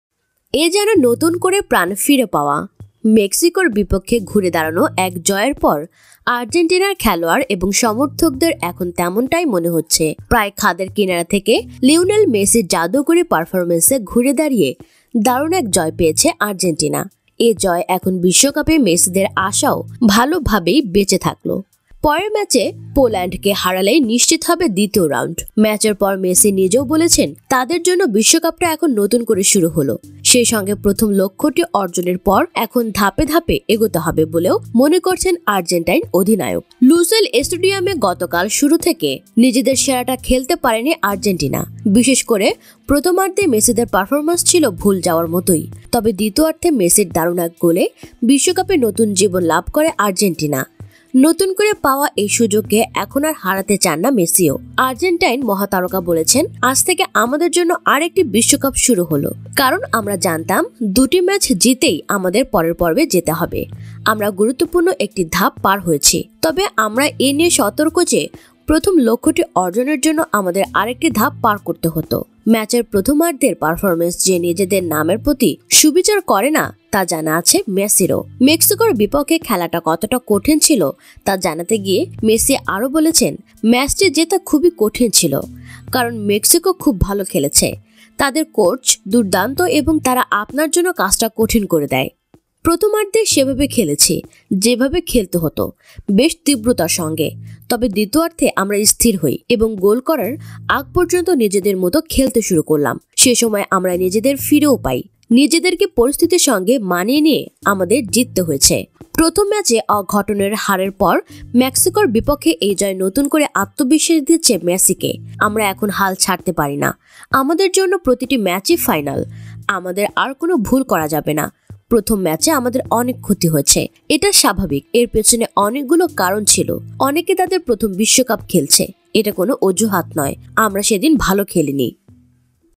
আজ থেকে আমাদের জন্য আরেকটি বিশ্বকাপ শুরু হলো - Messi
এ যেন নতুন করে প্রাণ ফিরে পাওয়া। মেক্সিকোর বিপক্ষে ঘুরে দাঁড়ানো এক জয়ের পর আর্জেন্টিনার খেলোয়াড় এবং সমর্থকদের এখন তেমনটাই মনে হচ্ছে। প্রায় খাদের কিনারা থেকে লিওনেল মেসির জাদুকরি পারফরম্যান্সে ঘুরে দাঁড়িয়ে দারুণ এক জয় পেয়েছে আর্জেন্টিনা।

শীর্ষের সেরা ১০ টি জম্বি মুভি যা বক্স অফিসে এখন পর্যন্ত সবার উপরে । দেখে নিন কোন মুভি গুলো